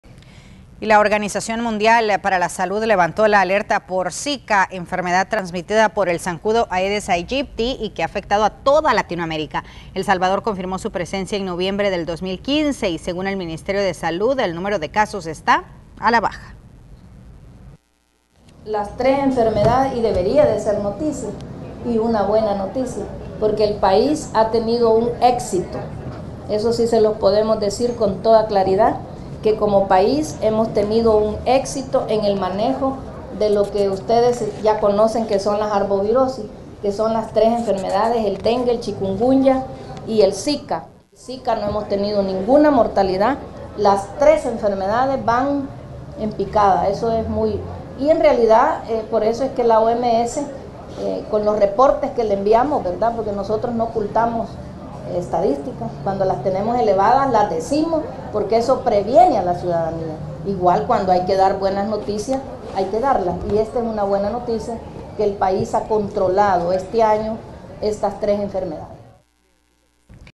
OMS deja sin efecto alerta por Zika
Ministerio de Salud asegura que funcionó campaña de prevención entre la población contra el zancudo portador de la enfermedad. Número de infectados en el país están a la baja